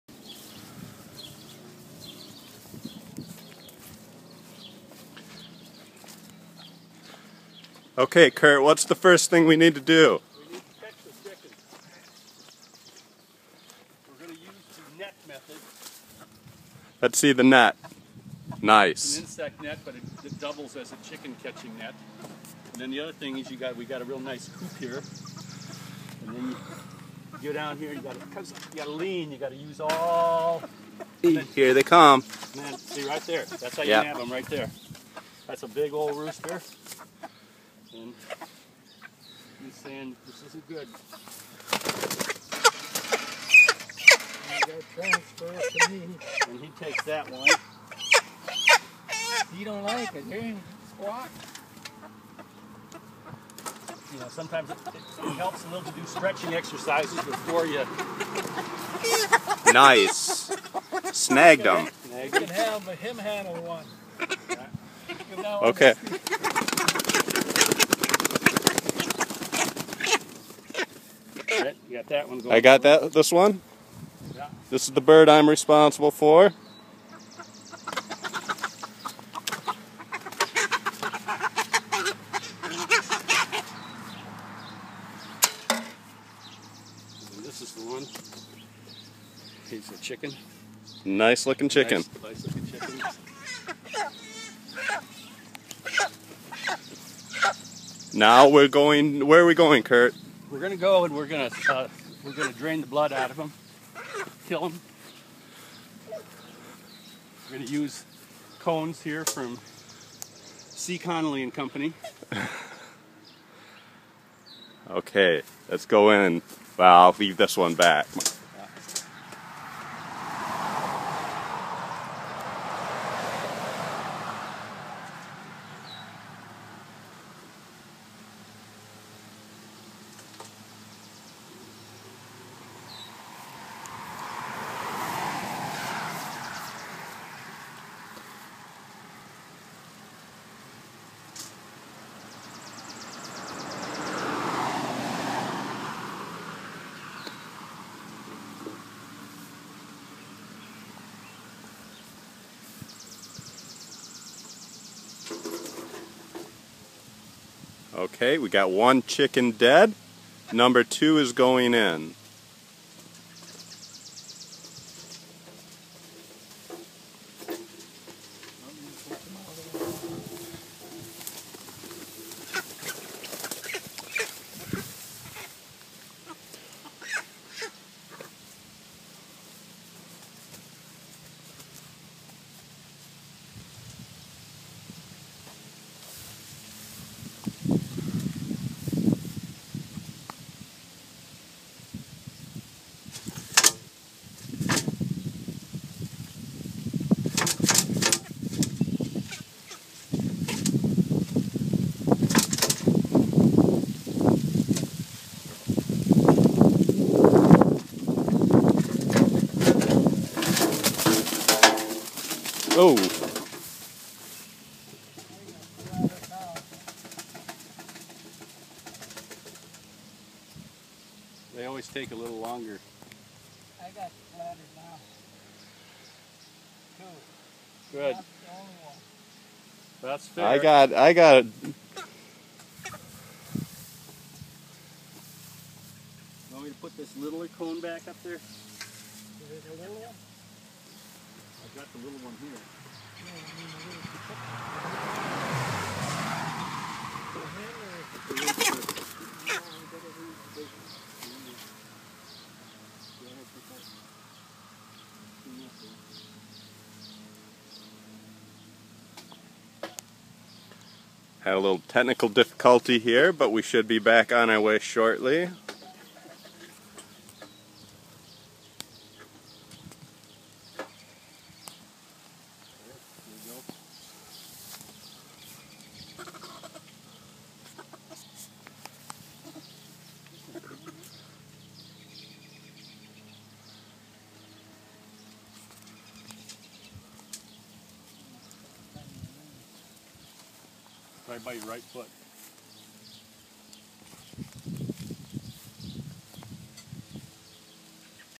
How to Butcher a Chicken
One in a two part series of how to turn a living chicken into a delicious dinner.